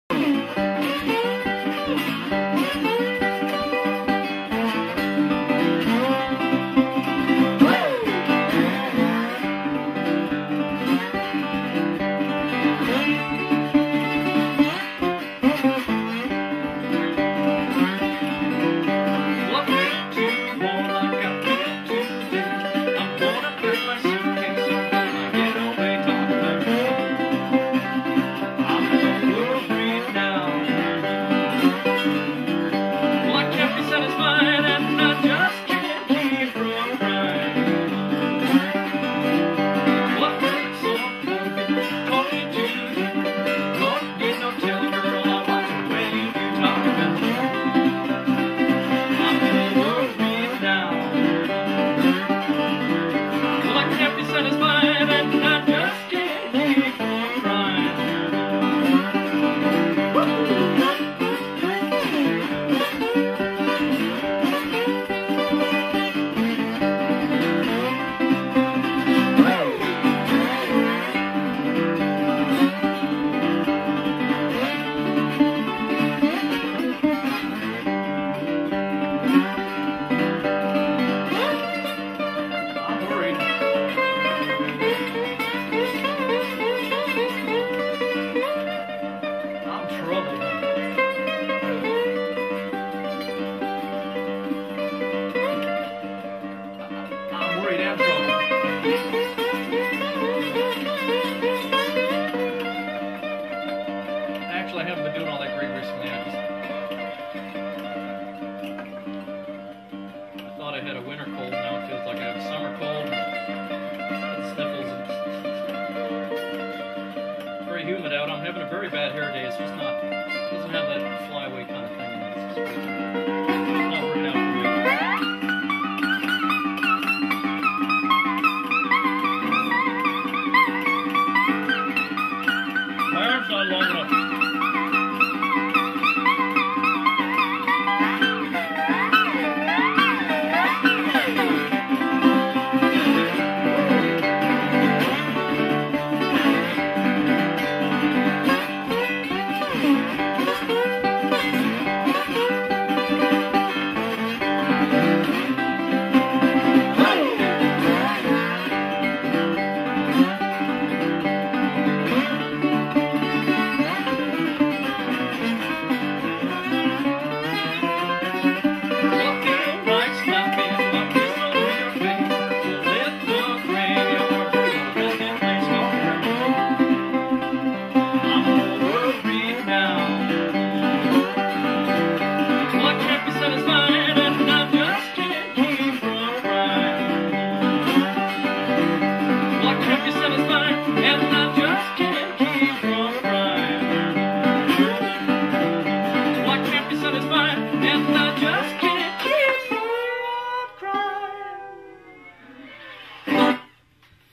David LaFleur plays in Pottsville
Folk singer David LaFleur played at All Things Good in Pottsville, PA in November 2006, enjoy his slide guitar technique..